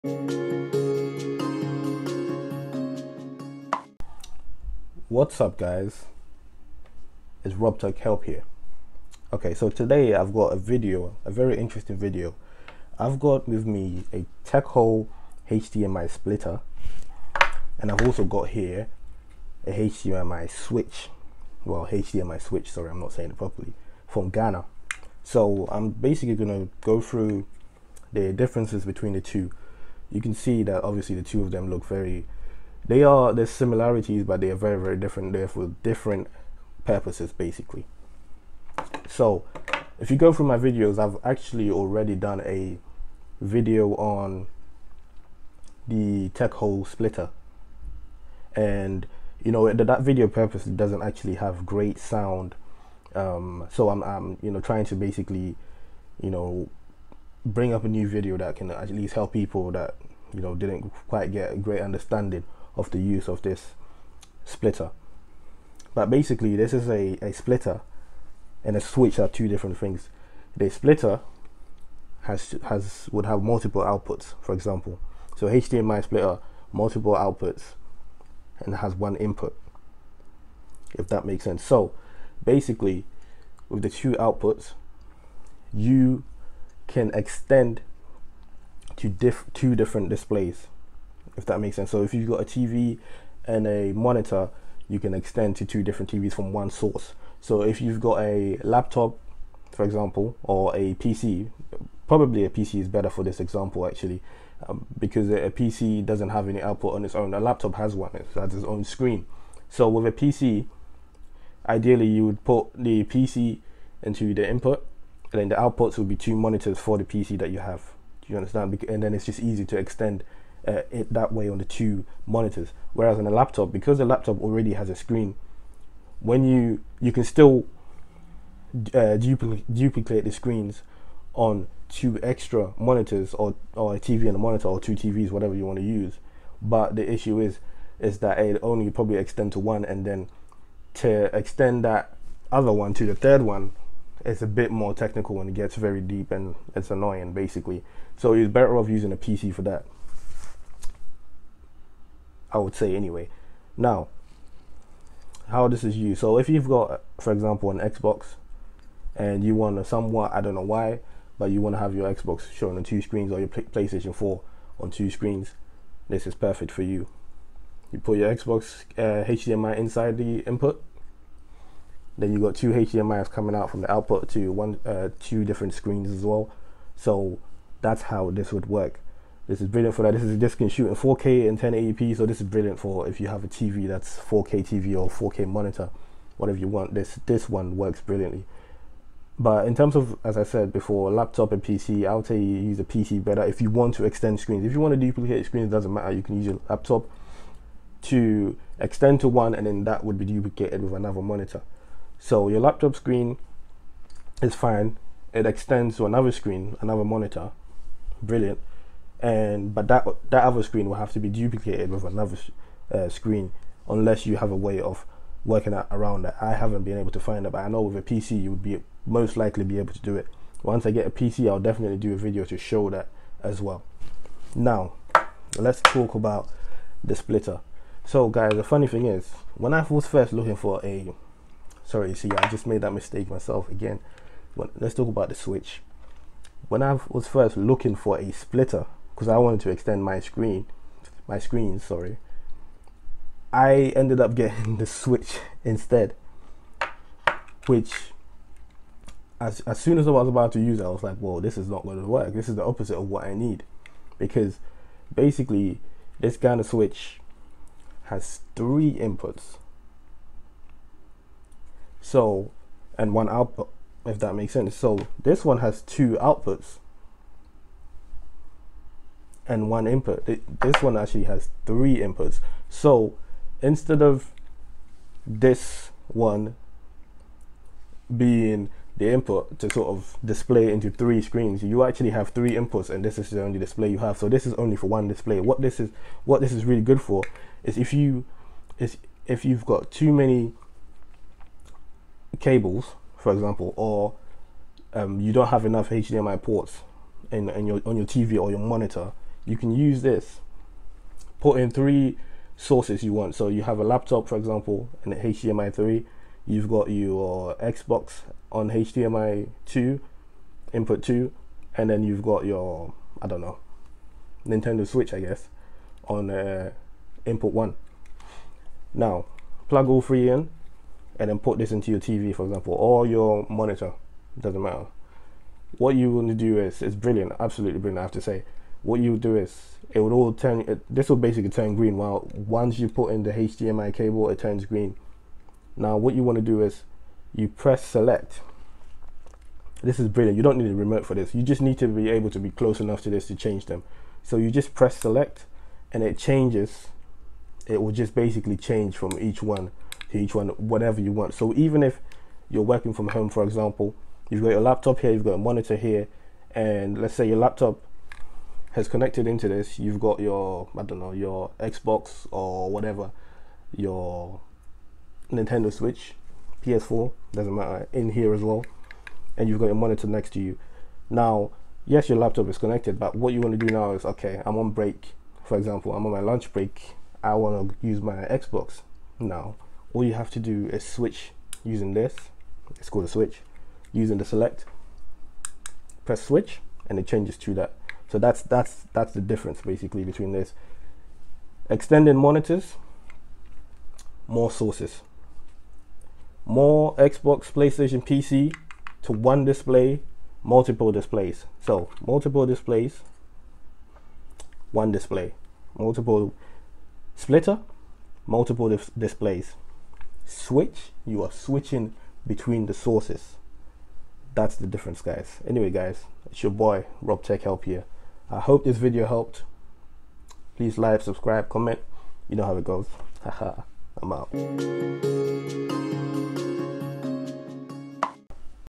Techole HDMI Splitter VS Gana HDMI Switch. Difference between a Switch and Splitter.
Hi Guys.  In this video I explain the differences between using a hdmi splitter and a hdmi switch, They are both very useful devices that can be used for specific things. Please watch and enjoy.

If you would like to purchase any of these please use the below links, it really helps the channel out.

HDMI Switch

HDMI Splitter